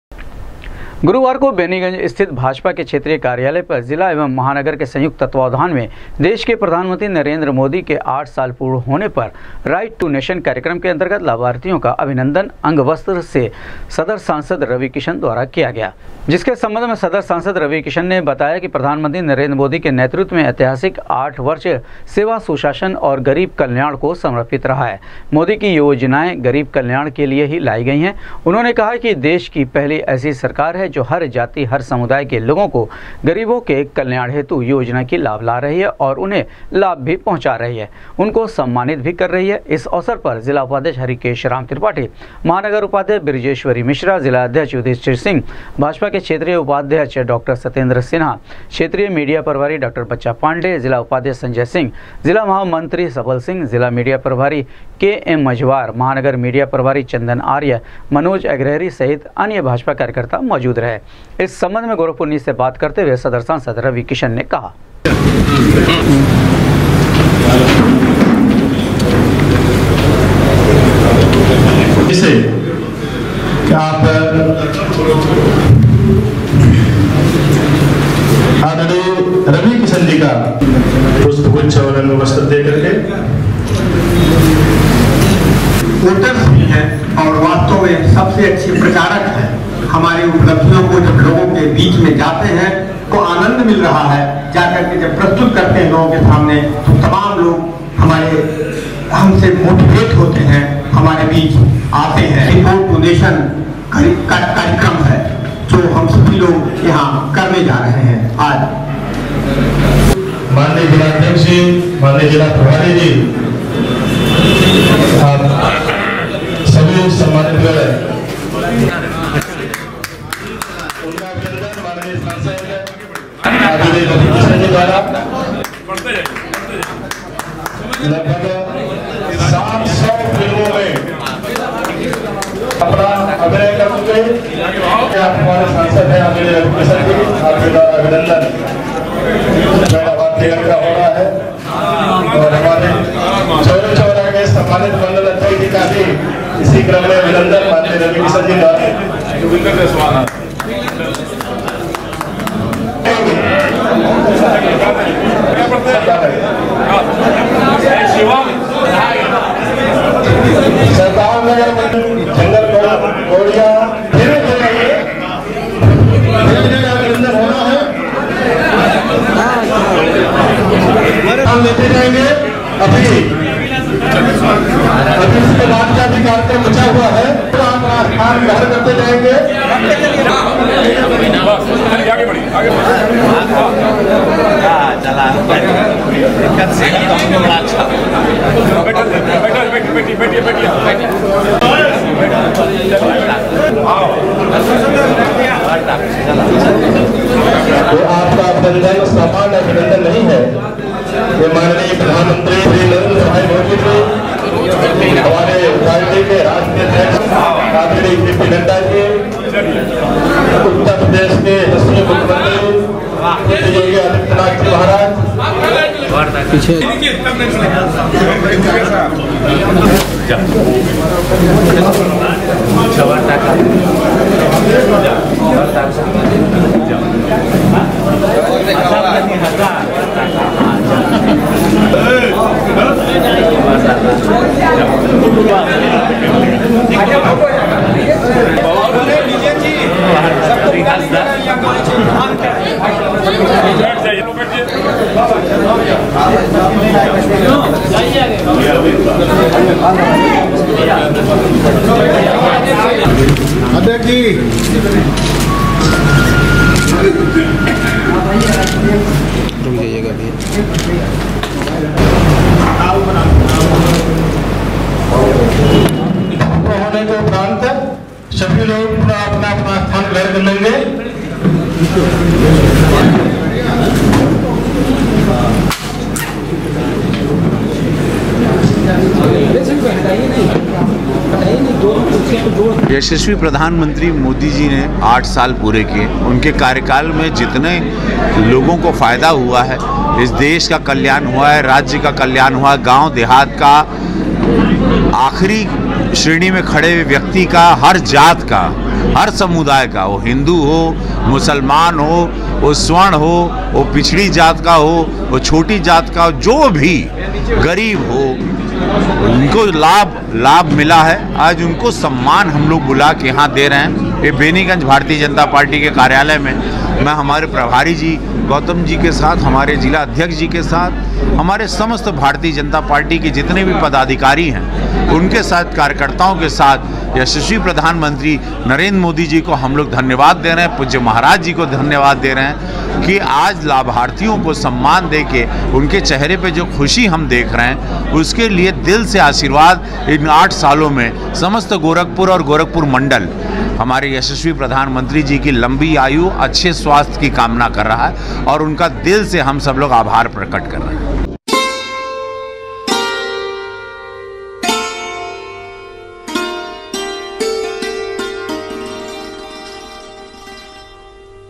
प्रधानमंत्री नरेंद्र मोदी के 8 साल पूर्ण होने पर राइट टू नेशन कार्यक्रम के अंतर्गत लाभार्थियों को
प्रधानमंत्री नरेंद्र मोदी के 8 साल पूर्ण होने पर राइट टू नेशन कार्यक्रम के अंतर्गत लाभार्थियों को सदर सांसद रवि किशन ने अंग वस्त्र प्रदान कर किया अभिनंदन,कहा, प्रधानमंत्री नरेंद्र मोदी के नेतृत्व की ऐतिहासिक 8 वर्ष की सरकार, सेवा,सुशासन और गरीब कल्याण को रही है समर्पित